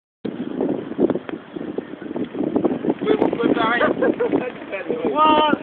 Sand yachting
A French lady experiences sand yachting for the first time on a beach at Boulogne-sur-Mer, France.